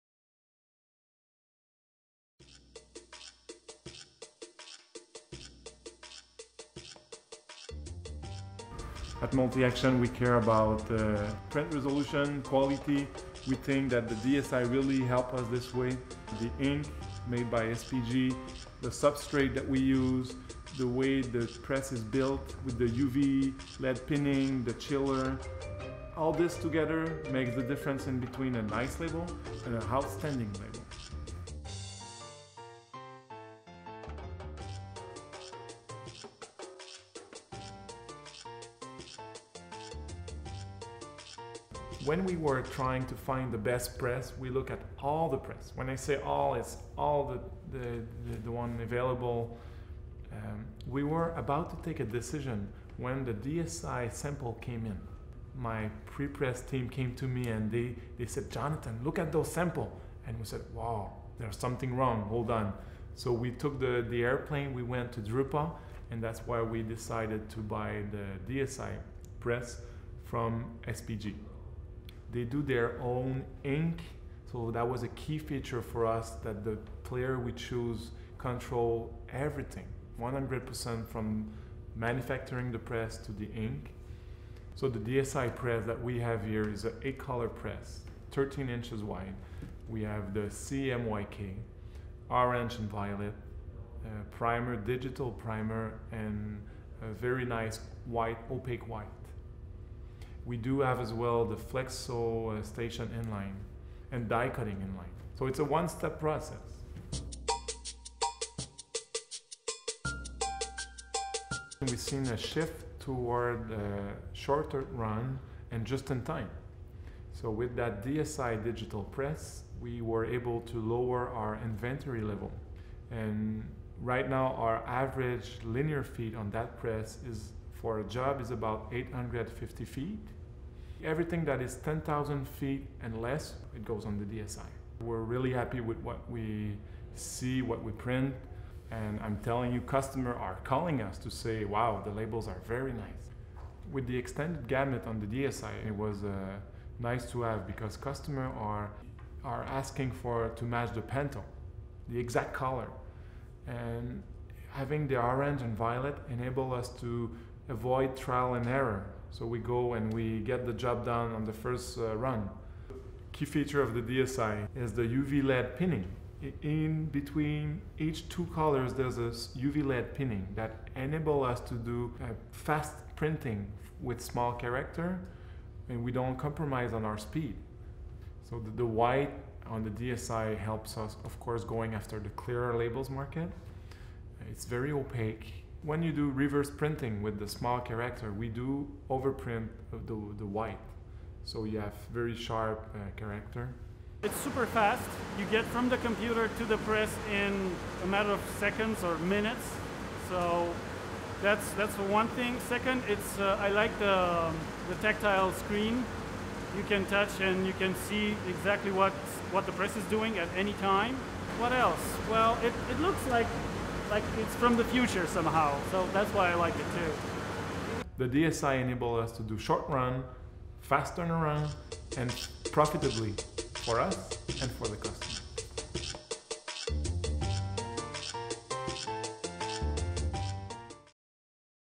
Multi-Action Labels achieves short-run success with SPGPrints' DSI® UV inkjet printer
A 10-colour DSI® UV-inkjet printing and converting press from SPGPrints is empowering Multi-Action Labels (Quebec City, Canada) to offer short runs of self-adhesive labels 'just-in-time' to customers in the retail, food & beverage, home care and industrial markets. As company president Jonathan Bourbonnière explains, the DSI stood apart thanks to its outstanding quality, modularity and high specification. Look out for a full interview, published next month!